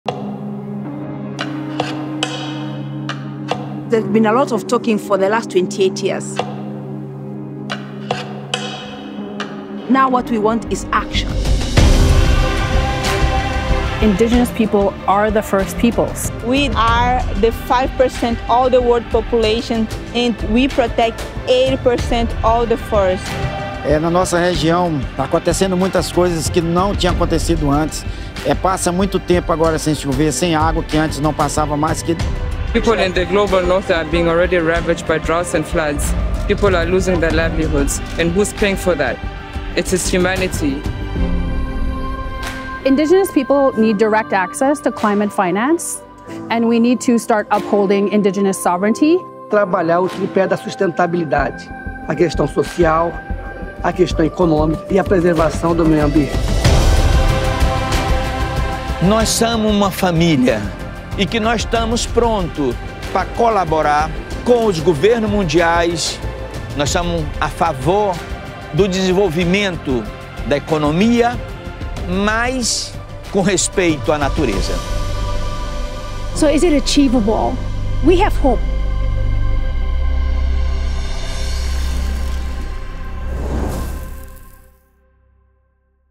COP28 Day 6 Snapshot: Indigenous Peoples
🌍 COP28 Snapshot – Day 6 – Energy, Industry, and Just Transition

🛡 Indigenous Peoples called on world leaders to protect ancestral land under threat from global warming, mining, oil and gas extraction.

 🇪🇨ECUA Indigenous Peoples community called for safeguards for people whose lands bear the brunt of climate change and exploitation of natural resources.

🌱 International Union for Conservation of Nature, International Indigenous Forum on Biodiversity, and IUCN Indigenous Peoples Organisations members launched the Podong Indigenous Peoples Initiative.

 💰 Mobilization of $200 million from 2023 to 2030 in Biodiversity and Climate finance, with 85% funds reaching Indigenous territories and local communities.